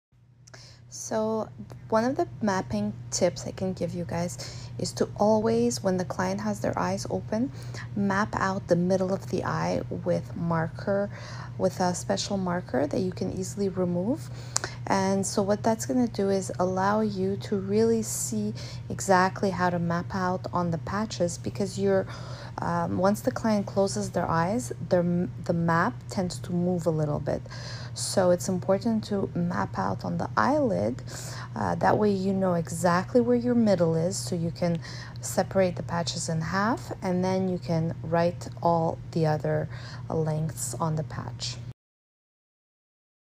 #1 Lash Mapping Hack
As a beginner lash artist, you might ask yourself many questions about mapping. What is lash mapping? How do you map eyelashes? Why is lash mapping essential?